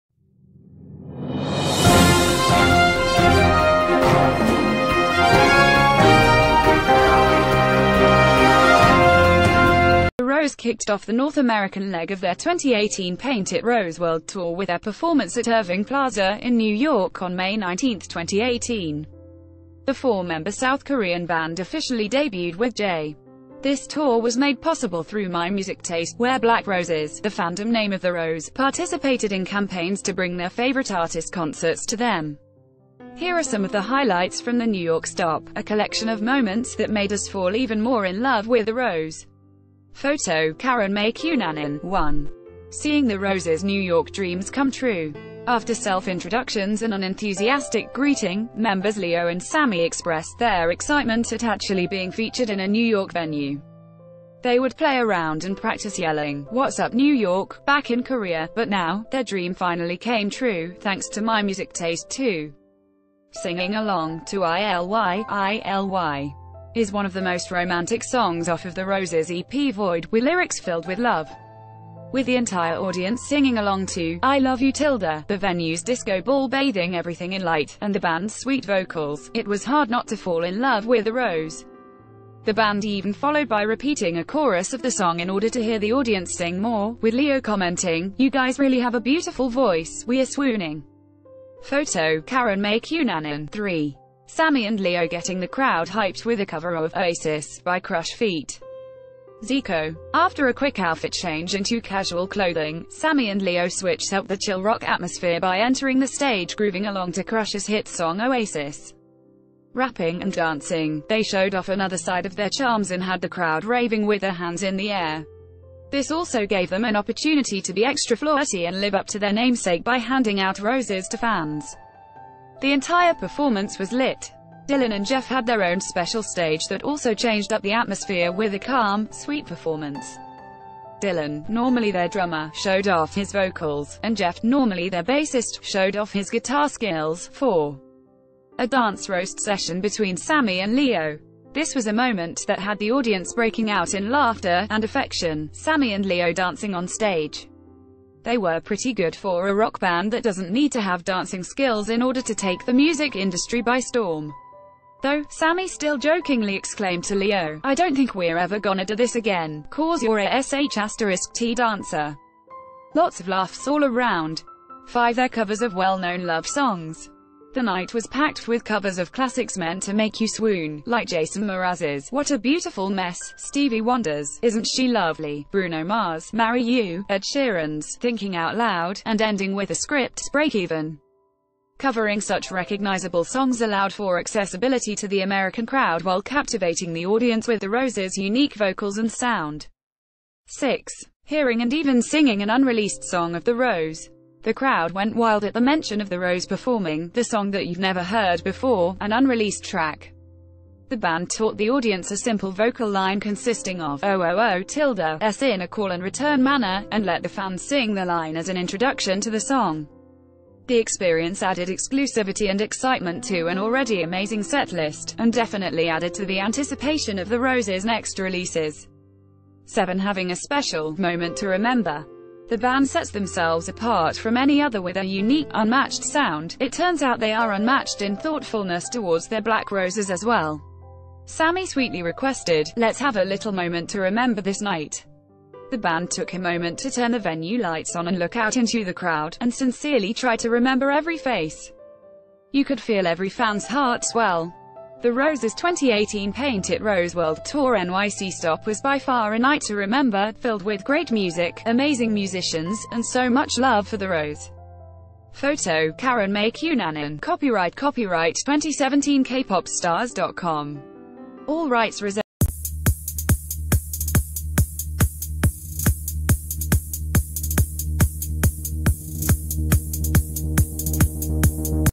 Moments We Love from The Rose's 2018 Paint it Rose World Tour (REVIEW)
The Rose kicked off the North American leg of their 2018 Paint it Rose World Tour with their perform...